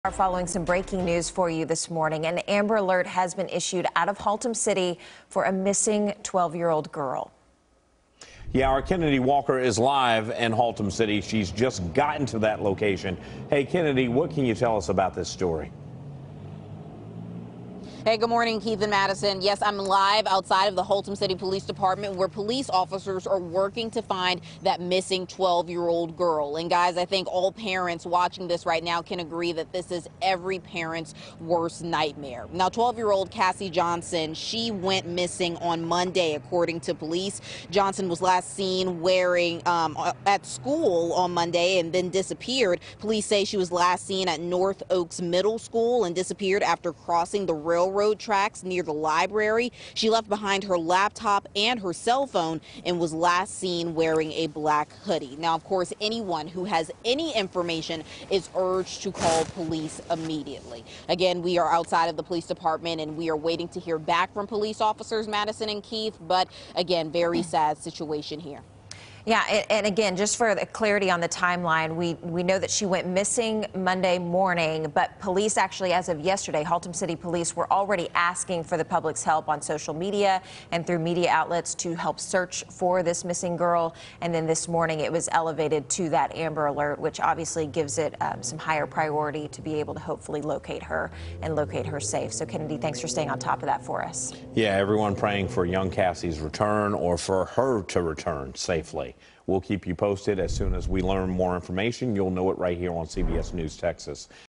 Amber Alert issued for 12-year-old Haltom City girl
Haltom City police are searching for 12-year-old Cassie Johnson. An Amber Alert was issued after she disappeared after crossing the railroad tracks by a library. She left behind her laptop and cell phone.